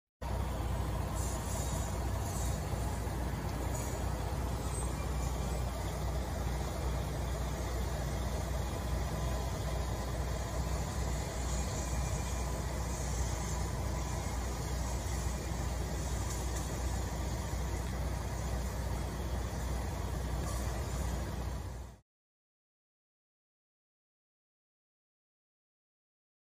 VIDEO NOW: Fire breaks out at flood-damaged Cranston apartment building
Dean Estates was recovering from flood damage from Sunday's heavy rain when a fire broke out Monday afternoon.